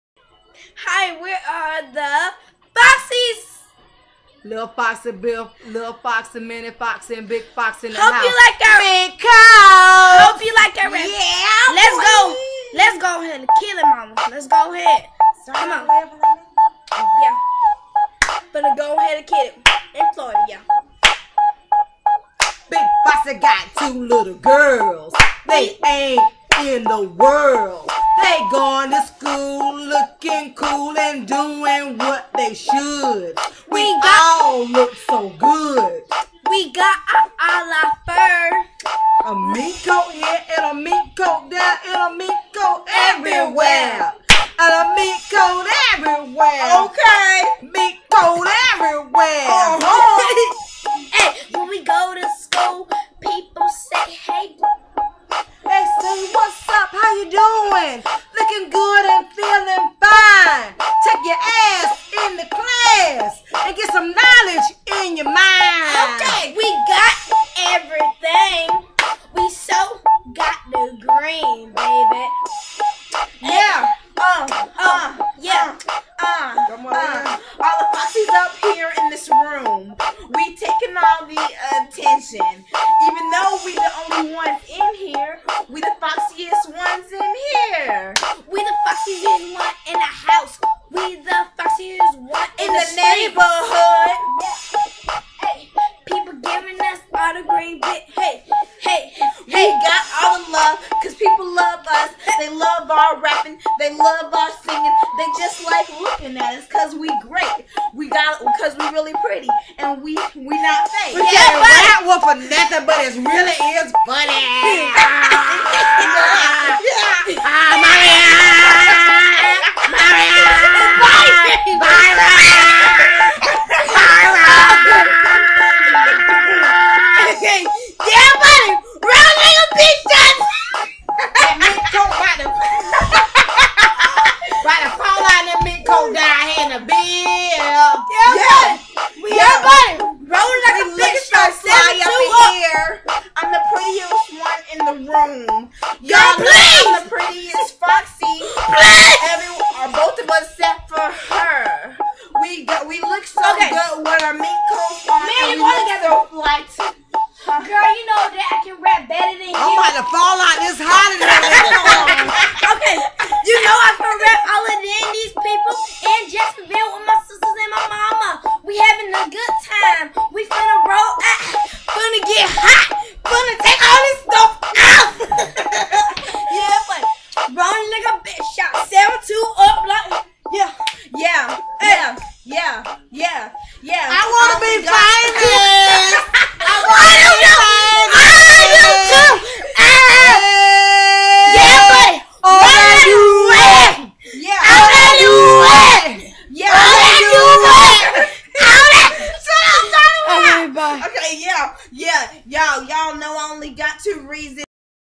The Foxxy Possie Minked OUt!
The foxxy possie  looking Fabulous in their Vintage Mink Coats!..Family Fun!!